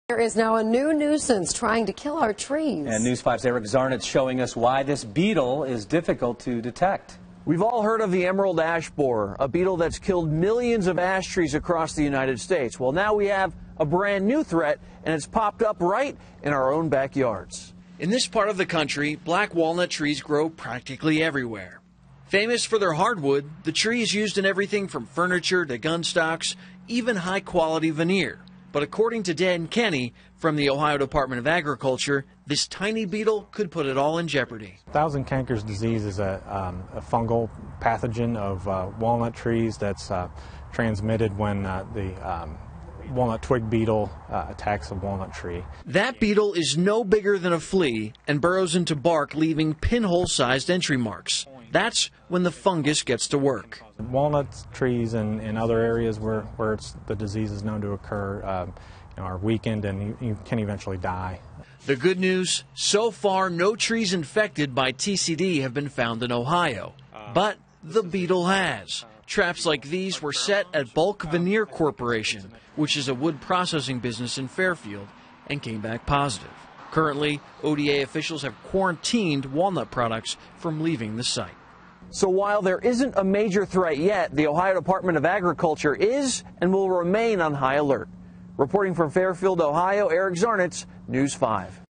Twig beetles pose threat to walnut trees
Walnut twig beetles can transmit the deadly fungal pathogen thousand cankers disease from tree to tree as they burrow into the bark, leaving pinhole-sized entry marks.